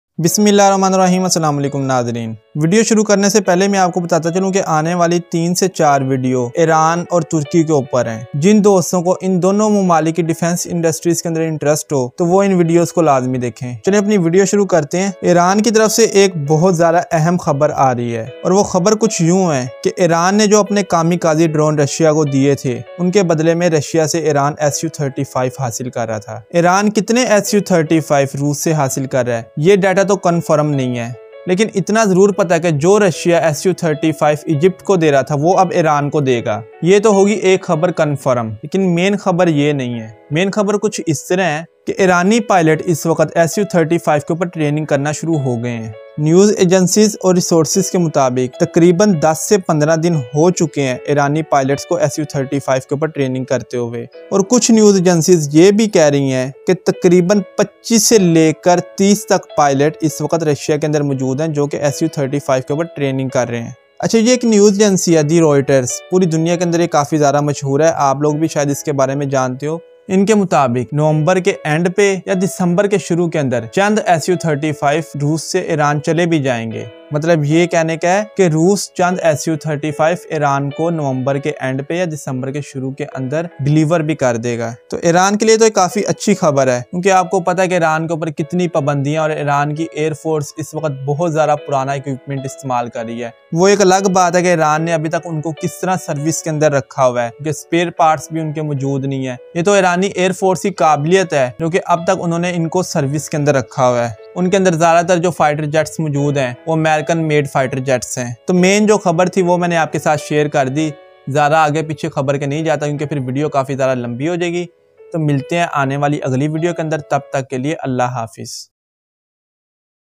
Iranian air force start training in Russia | Russia give SU 35 to iran in November | Ayyan Official
Iranian Air Force
Iran Buying SU 35
Russian SU 35
Iranian Air Force Buying New Fighter Jet
Iranian Air Force pilots training